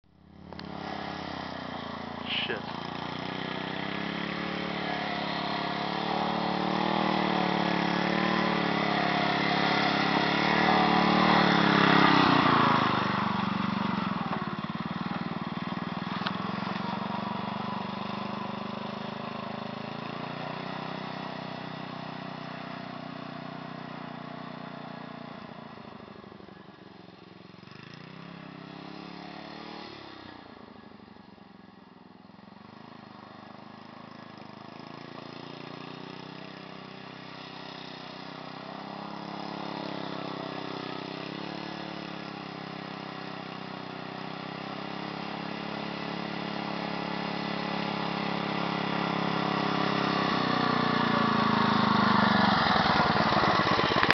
lawn tractor fun part 2
Me on my rally roper lawn tractor.